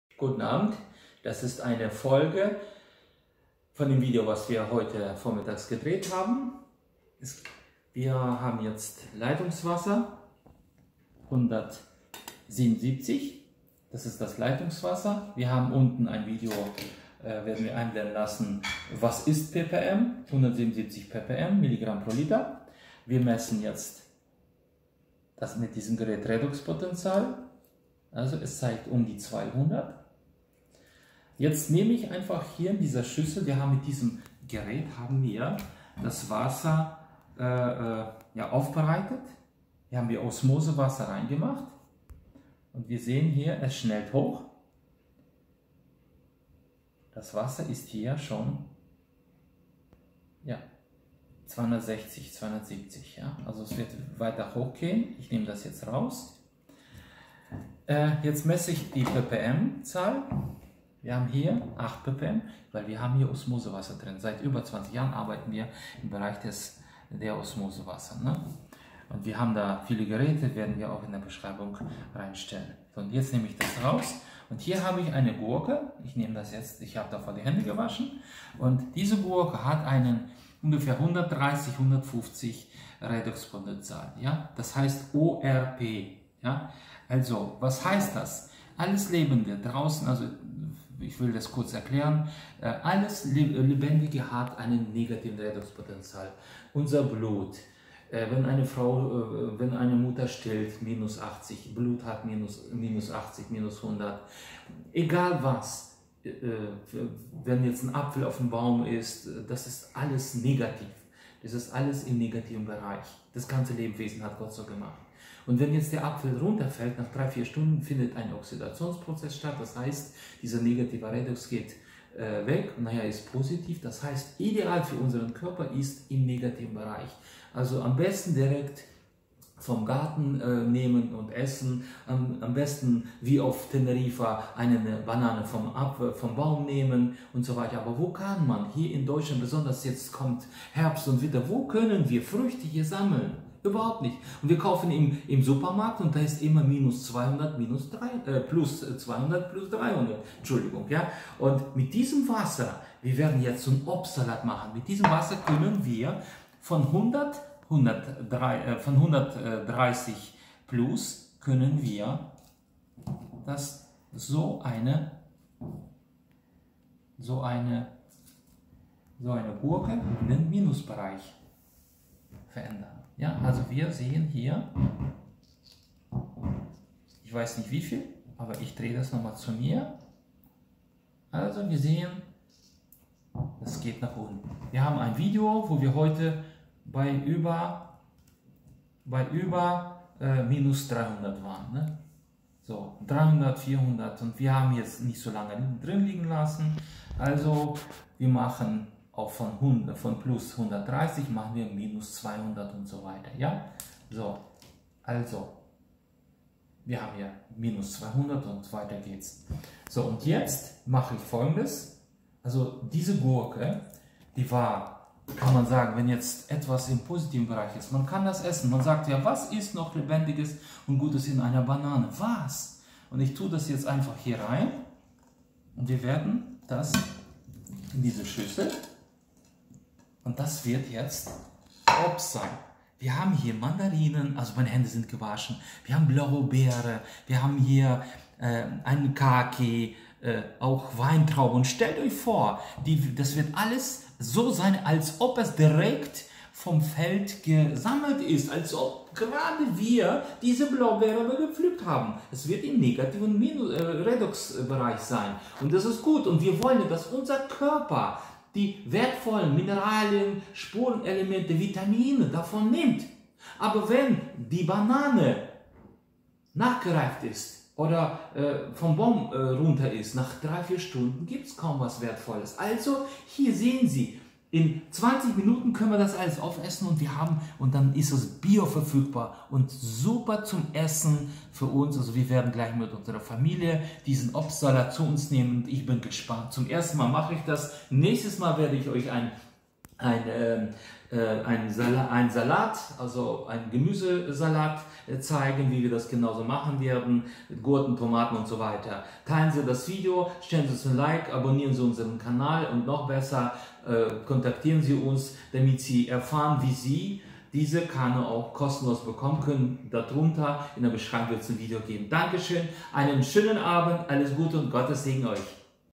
Zubereitung von gesundem Obstsalat mit Hilfe der Hydrogen Wasserquelle ( Hydrogenmaschine )hydrogen
Entweder haben Sie einen Filter, oder Sie (Ihre Nieren) sind der Filter / bitte bleiben Sie gesund🙏

einfach genial💧